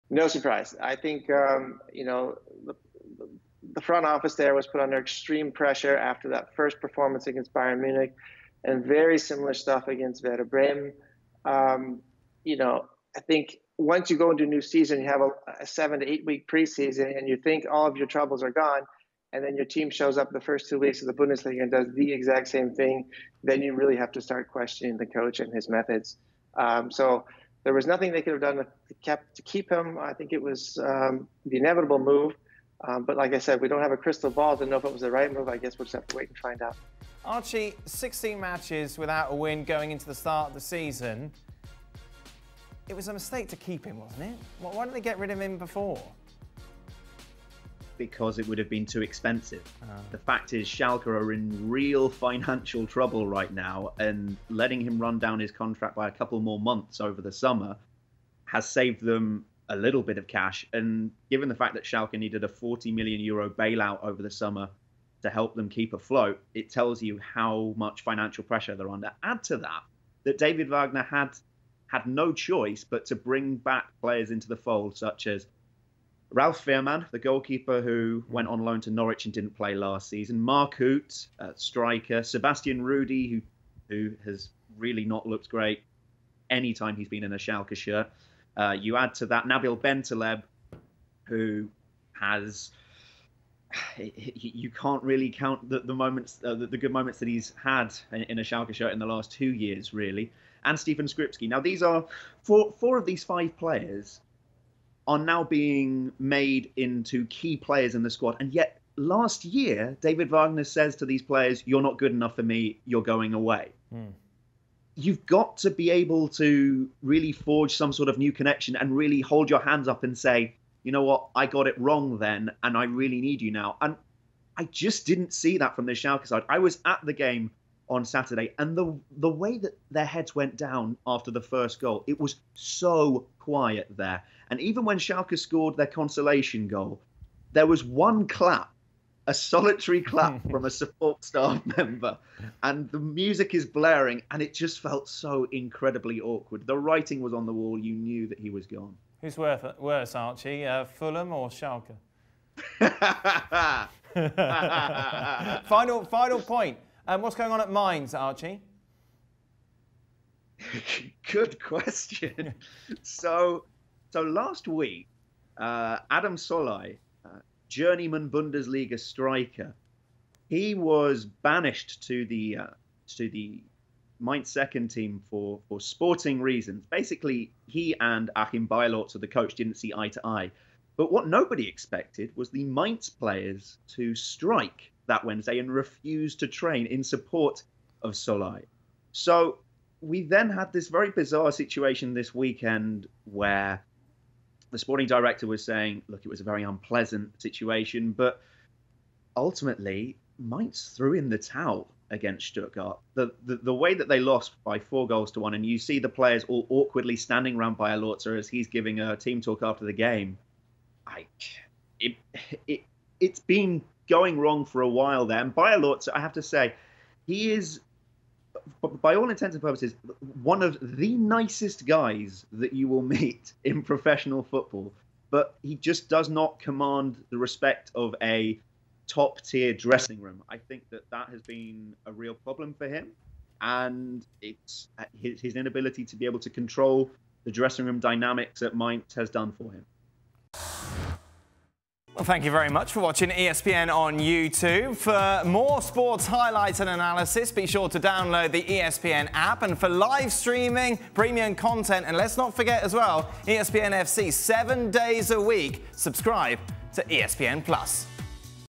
Two Bundesliga managers sacked in a day! What’s gone wrong at Schalke and Mainz? | ESPN FC
With Matchday 2 of the German Bundesliga claiming the first managerial casualties of the season, Steve Cherundolo joins ESPN FC’s Archie Rhind-Tutt and Dan Thomas to look back at where it all went wrong for American David Wagner at Schalke and Achim Beierlorzer at Mainz. Rhind-Tutt explains why financial disarray forced Schalke to hang on to Wagner through the summer, and why “nice guy” Beierlorzer couldn’t win over the Mainz dressing room and prevent a player strike after he suspended Adam Szalai from the senior squad.